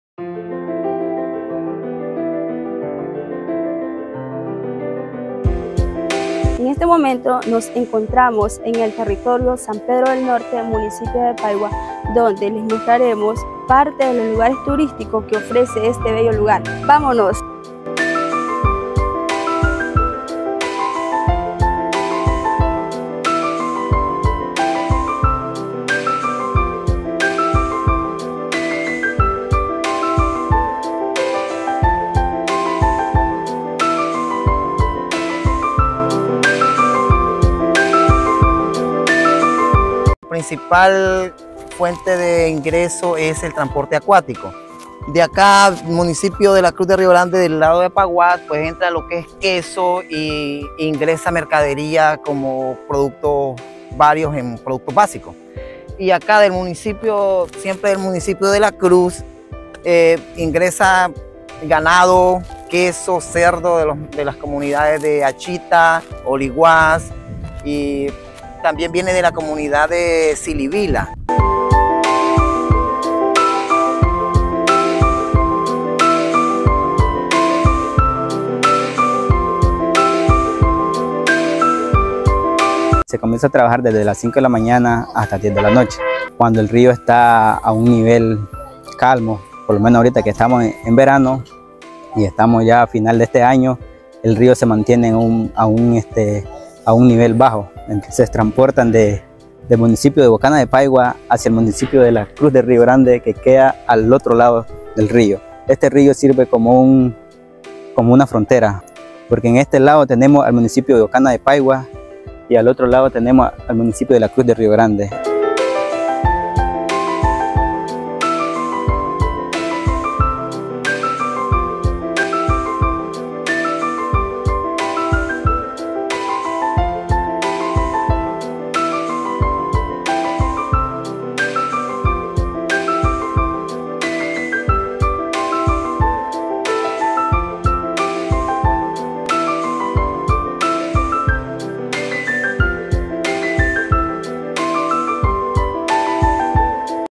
Explorando el Turismo Comunitario en San Pedro del Norte 🌴 en la Costa Caribe Sur de Nicaragua 🏞️🇳🇮
¡Acompáñanos en esta emocionante travesía por San Pedro del Norte, un rincón auténtico en la Costa Caribe Sur de Nicaragua! 🇳🇮✨ Exploraremos el turismo comunitario, sumergiéndonos en la cultura local, la belleza natural y la hospitalidad única de esta joya en el Municipio Bocana de Paiwas. 🏞️🌅

Te llevaremos en un viaje auténtico que solo la Red de Jóvenes Comunicadores puede ofrecer. 🎥🌐 ¡Prepárate para vivir la experiencia! Nicaragua TurismoComunitario CostaCaribeSur somosred 🚗🗺️